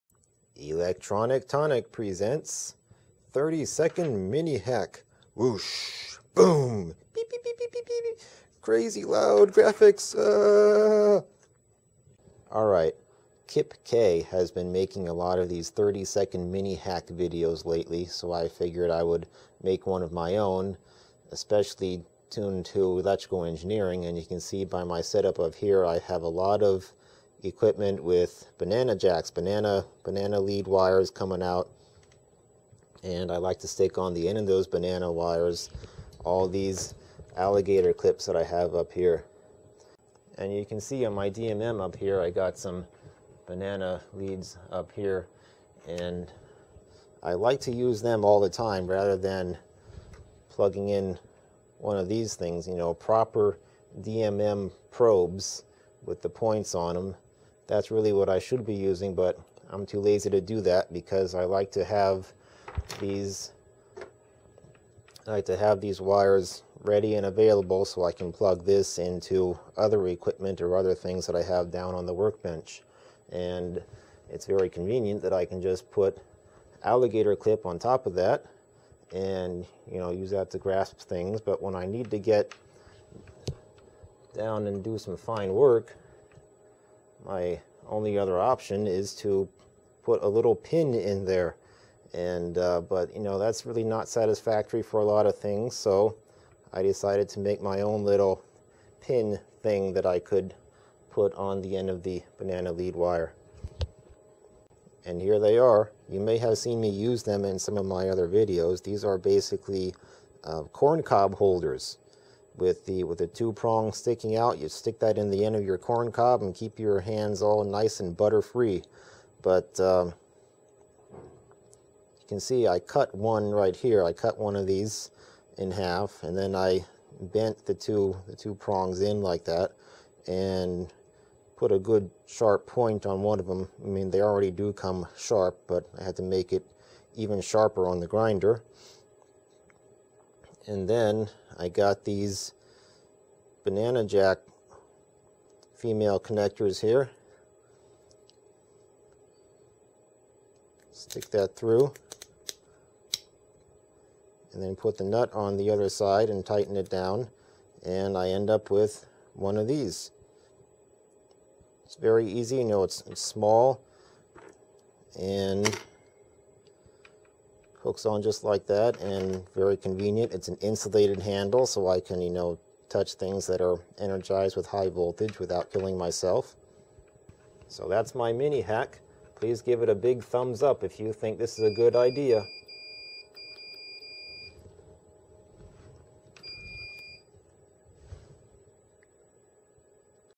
(#0063) 30-Second Mini Hack - Corn Cob Holders for DMM Probe Tips
An informative parody of KipKay's ":30 Mini Hack" videos.